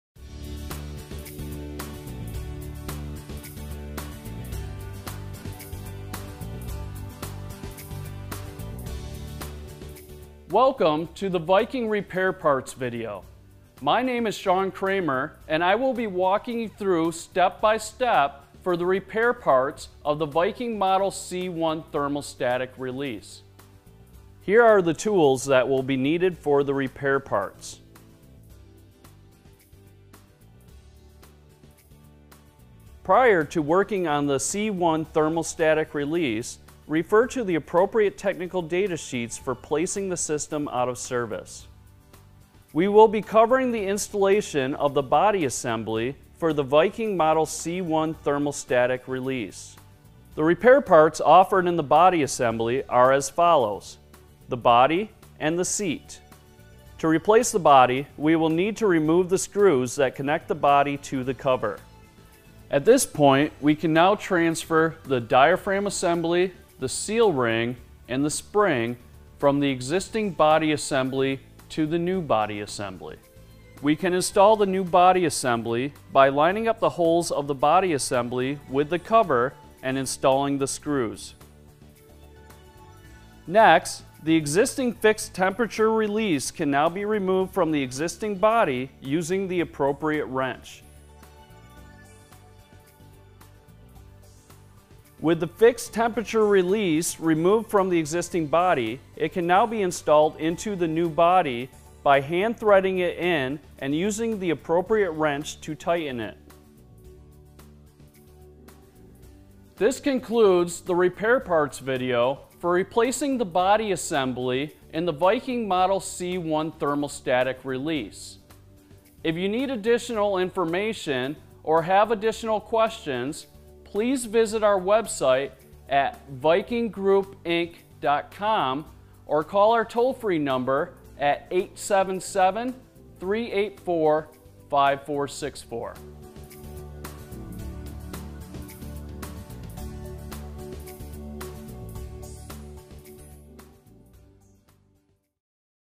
Viking Model C-1 Thermostatic Release Body Assembly
The Viking Model C-1 Thermostatic Release is a rate-of-rise releasing device for use on hydraulic and pneumatic release systems controlling the operation of Viking deluge valves on deluge or preaction systems.

About Viking